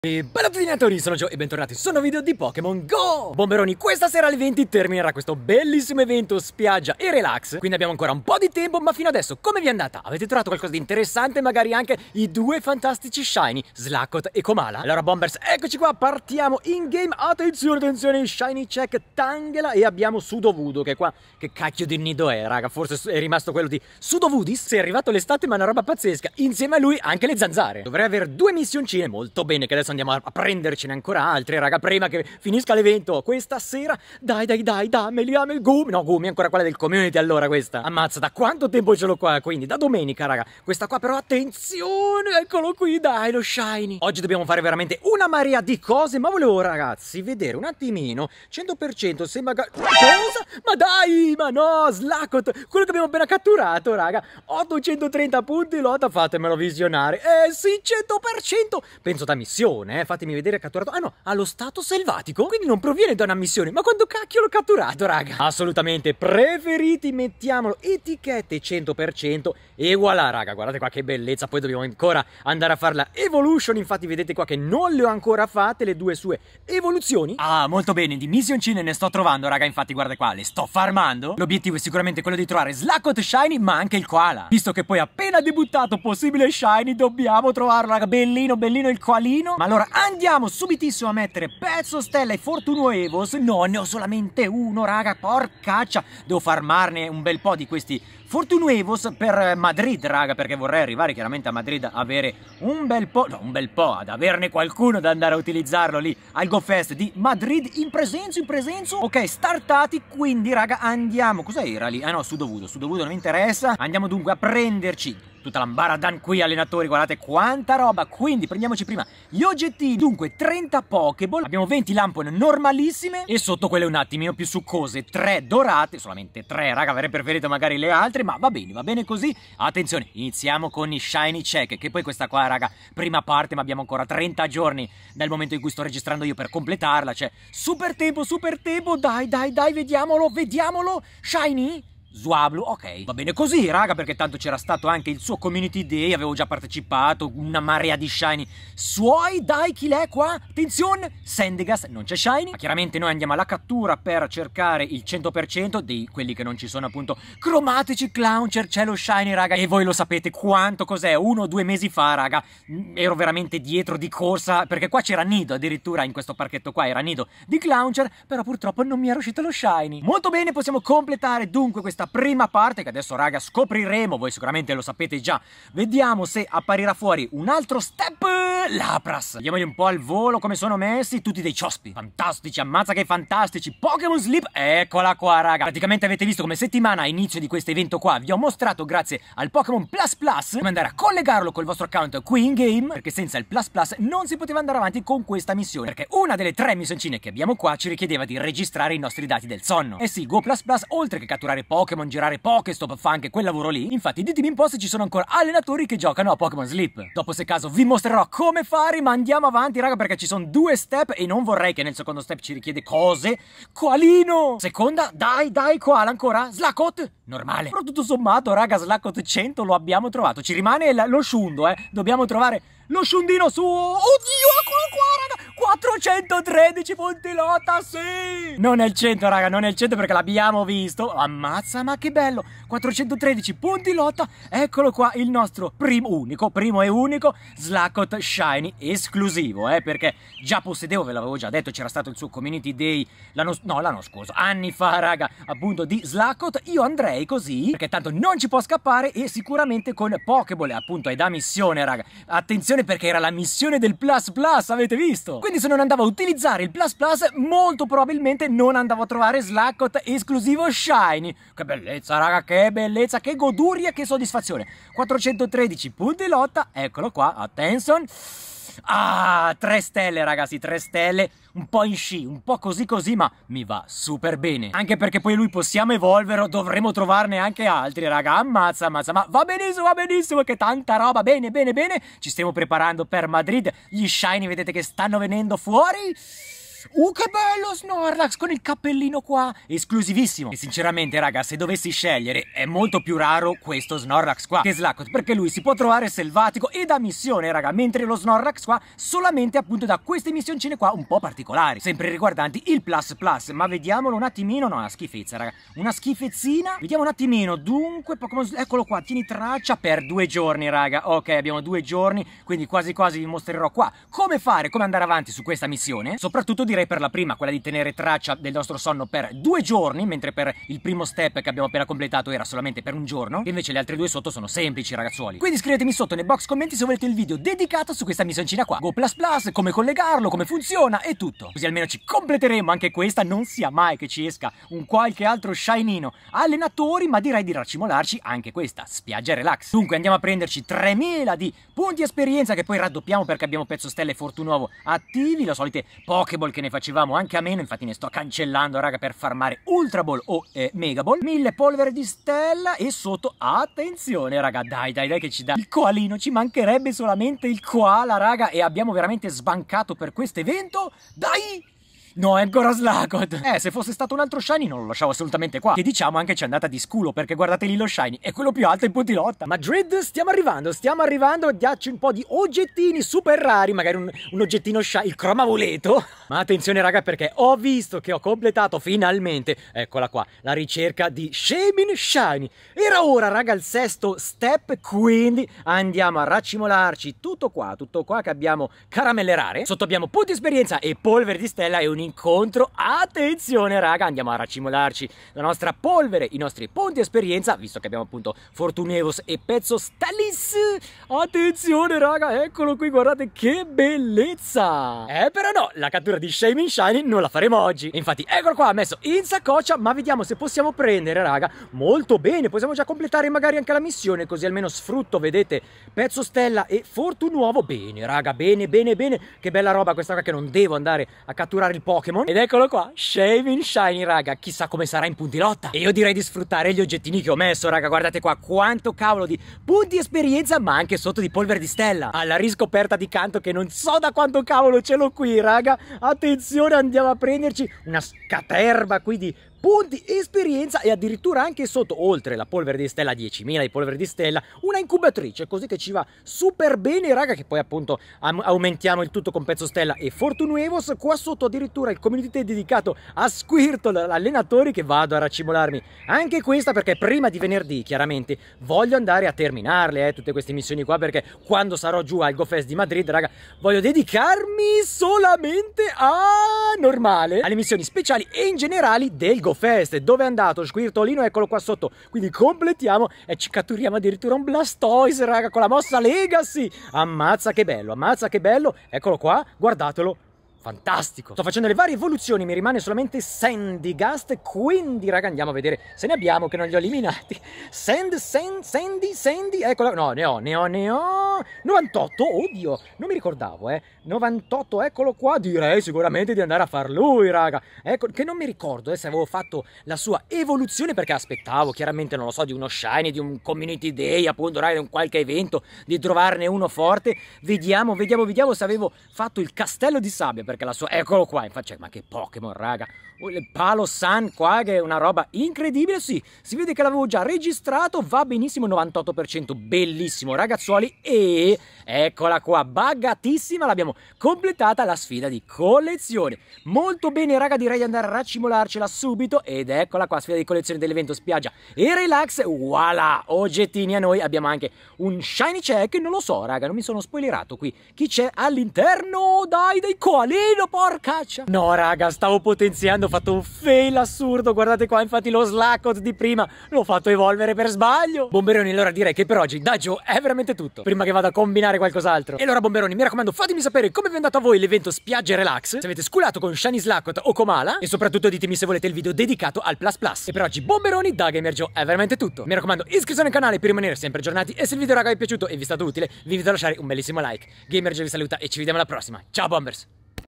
UNA SCULATA IPER GALATTICA!!! ✨😱🤯
Ciao a tutti allenatori!
Ultimo giorno di evento prima di partire per Madrid, profumo di shiny e sculata dietro l'angolo con shiny Slakoth esclusivo in più anche un bel 100%!
Come hai concluso l'evento?🍀
Al prossimo video, ciao!